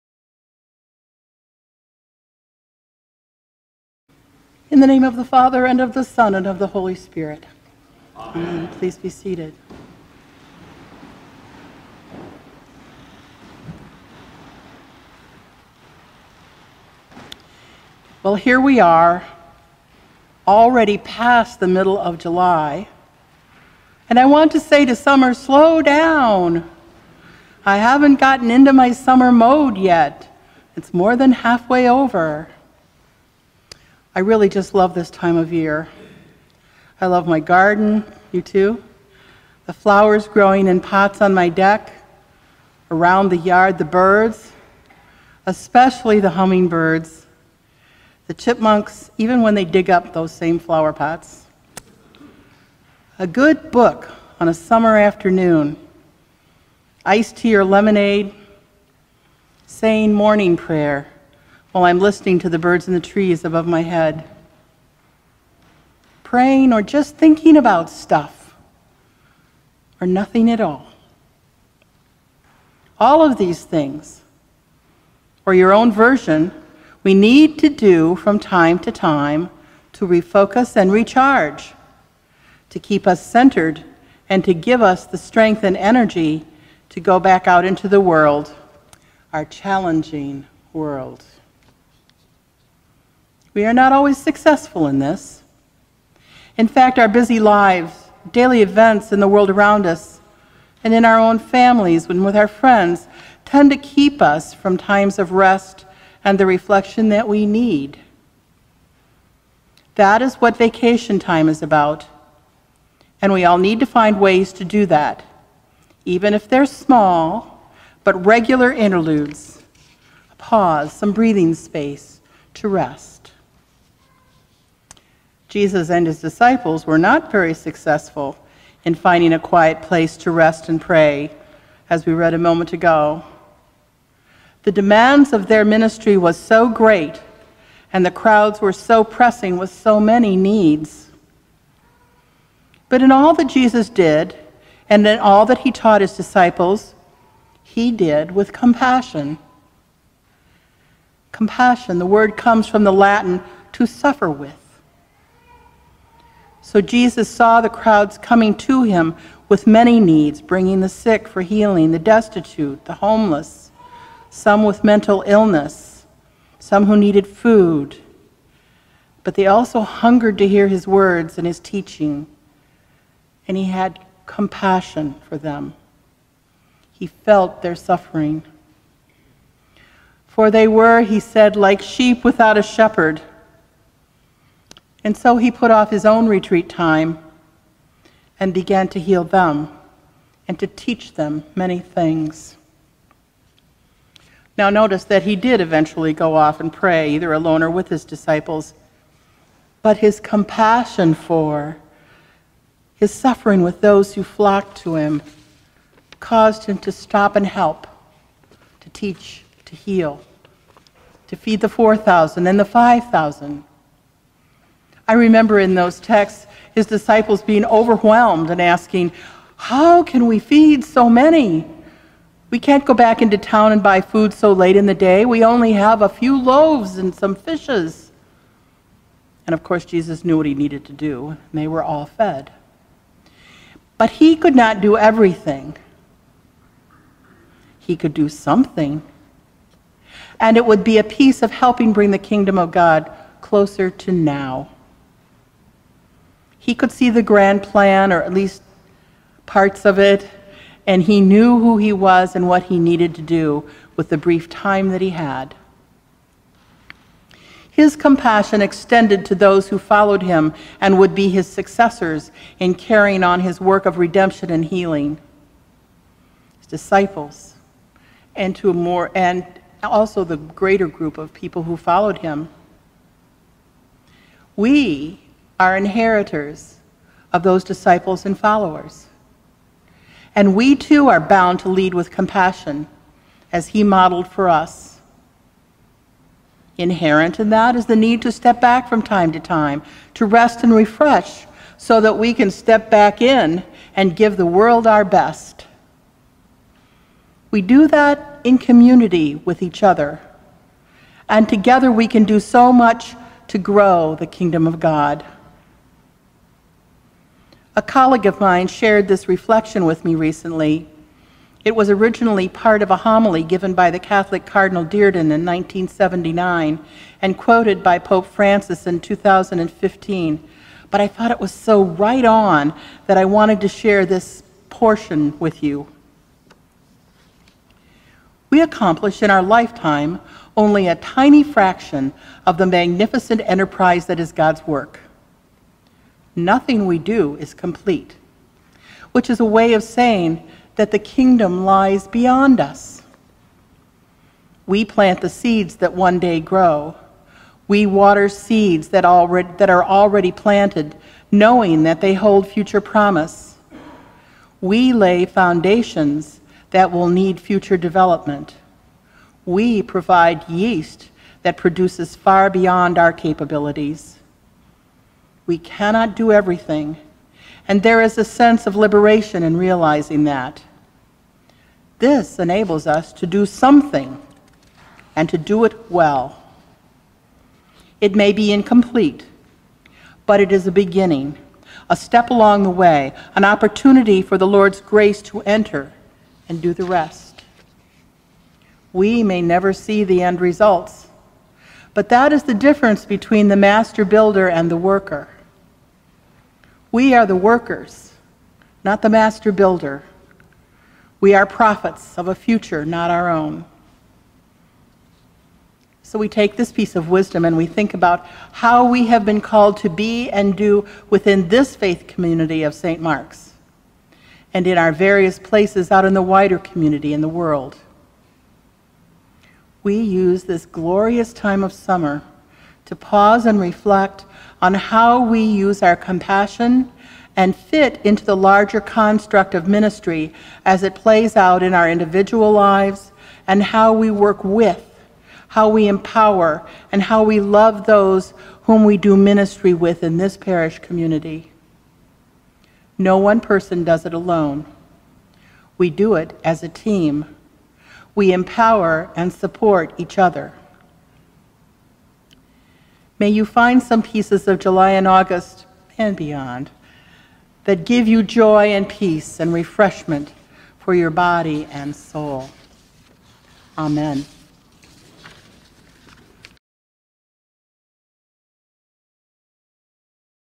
The Ninth Sunday After Pentecost Homily
This week's Scriptures are 2 Samuel 7:1-14, Psalm 89:20-37, Ephesians 2:11-22, and the Holy Gospel according to Mark 6:30-34,53-56.  Mother Sue delivers the homily on this Ninth Sunday after Pentecost on Sunday, July 22, 2018, St. Mark's Episcopal Church, Grand Rapids, Michigan.